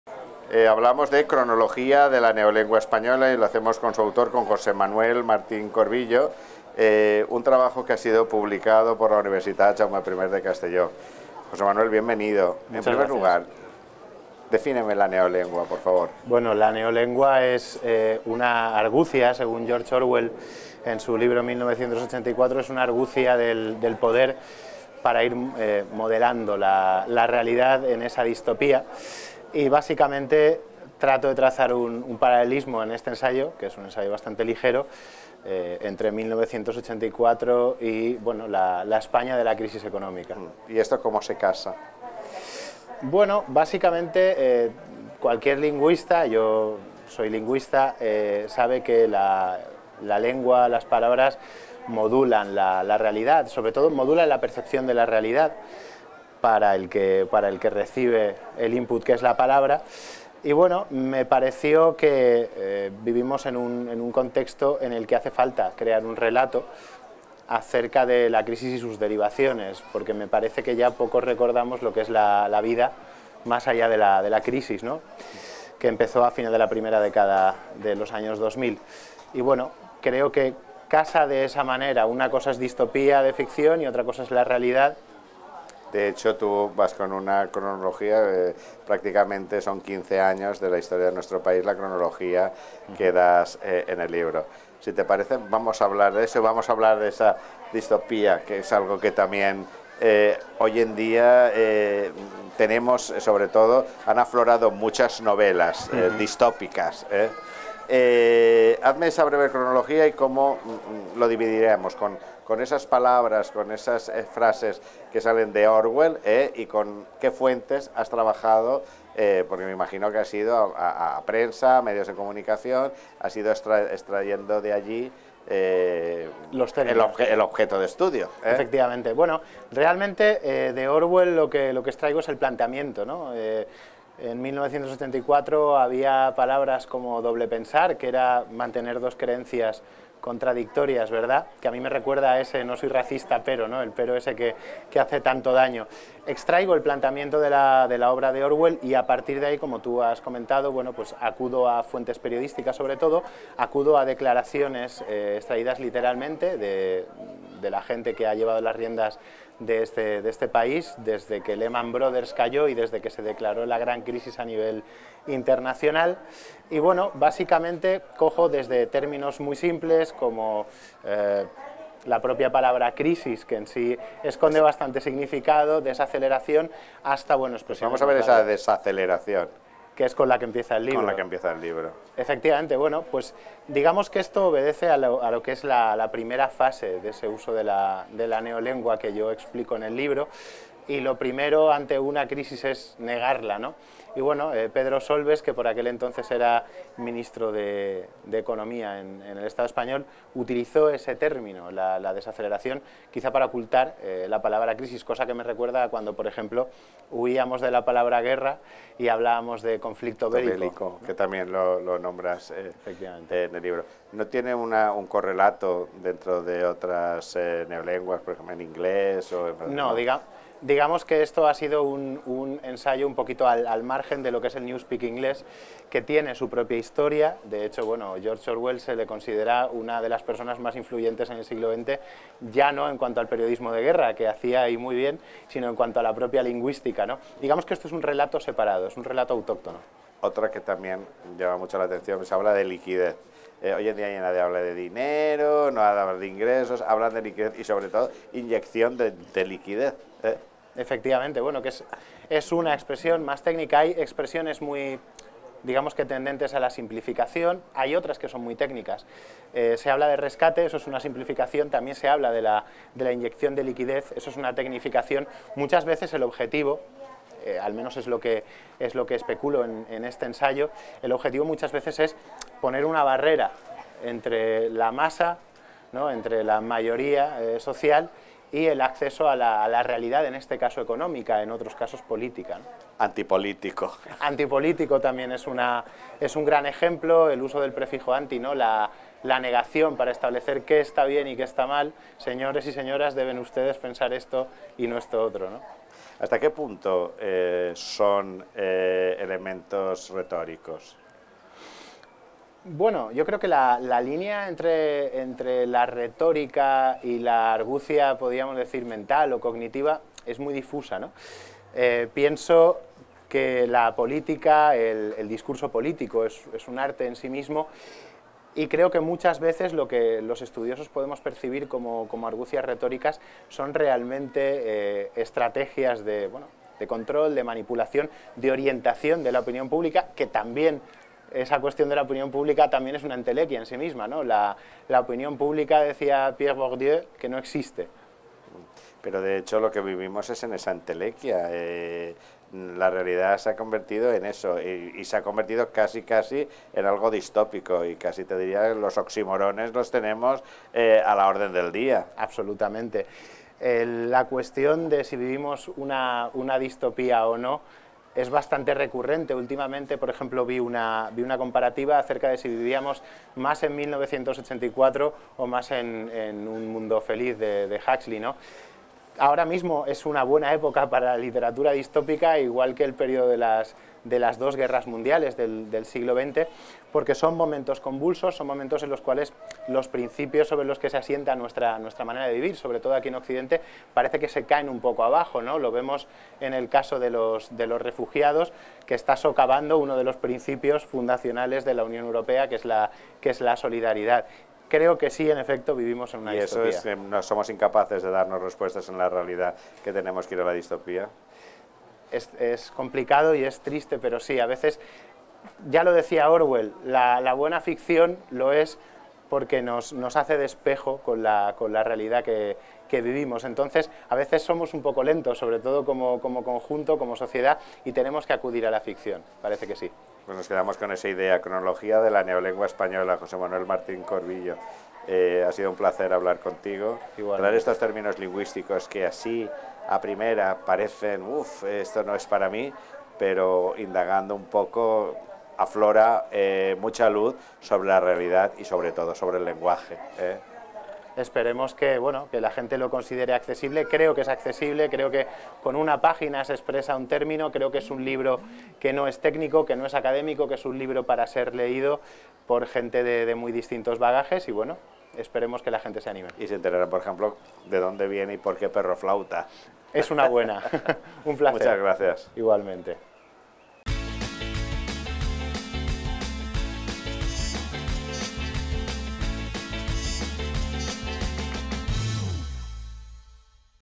Fira del Llibre 2016 | José Manuel Martín - “Cronología de la neolengua española”
Inspirat en la neolengua de 1984, l'obra de George Orwell, l'autor analitza prop de seixanta declaracions de governants i mitjans afins als poders fàctics des de 2008 fins a 2015. Aquest assaig accessible, àgil i àcid permet relacionar els termes subtilment introduïts des de l'esclat de la crisi en l'univers mental de la societat espanyola amb les decisions polítiques executades en aqueix temps. Decisions preses sota la pressió d'entitats no electes com a BCE, FMI o agències de qualificació de deute, i que han contribuït al fet que, en 2014, el 8,6 % de la població mundial posseïra més del 80 % de la riquesa segons el banc Credit Suisse.
Una reflexió sobre el llenguatge emprat per legisladors i mitjans per a respondre a una pregunta essencial: és la crisi un fet accidental o, per contra, és una conjuntura generada i aprofitada per a legitimar un canvi de règim a favor de les grans corporacions?